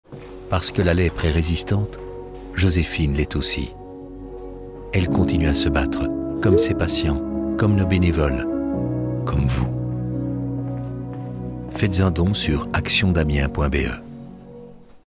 La lèpre est résistante, nous aussi ! | Campagne Action Damien 2022
Parce que la lèpre est résistante, Joséphine l’est aussi.
Abandonner ? Jamais.
Elle continue à se battre. Comme ses patients, comme nos bénévoles...comme vous.
Rejoignez-nous, faites un don sur actiondamien.be.